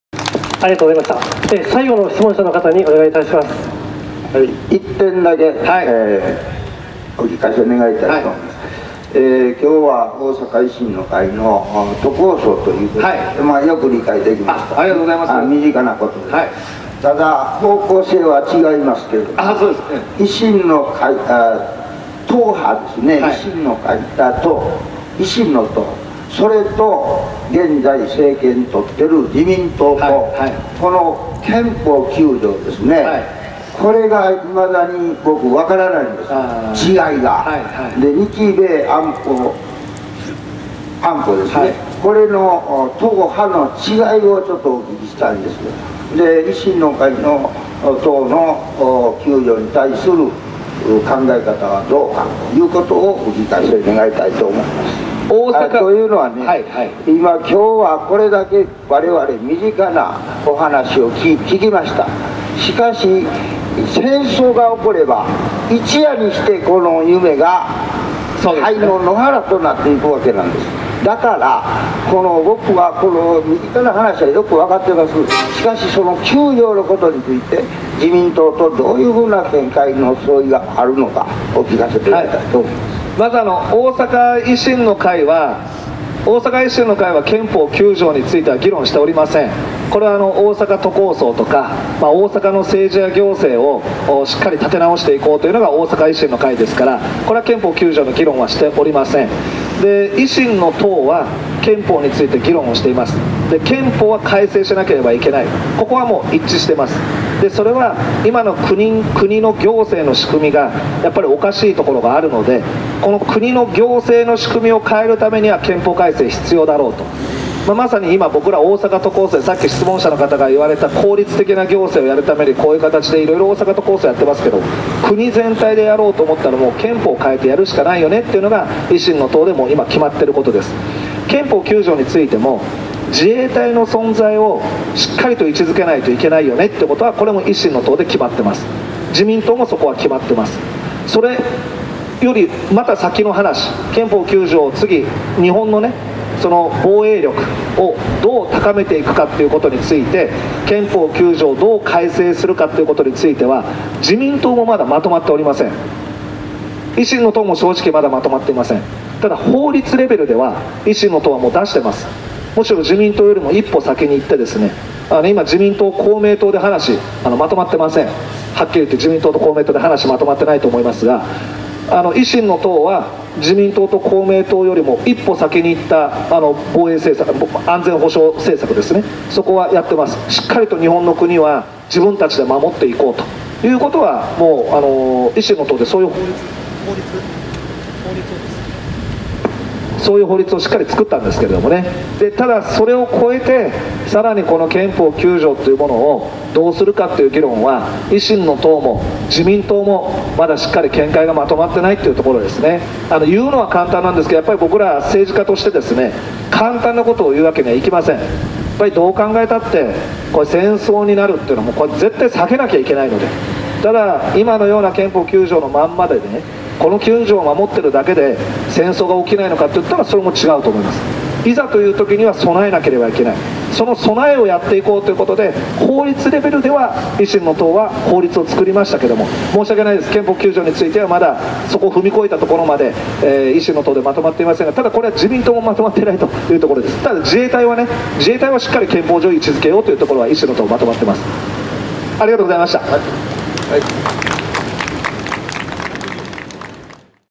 橋下徹、大阪市民『維新の党の憲法９条に対する考え方はどうか？』
橋下　大阪維新の会　維新の党
憲法9条　日米安保
2015年03月31日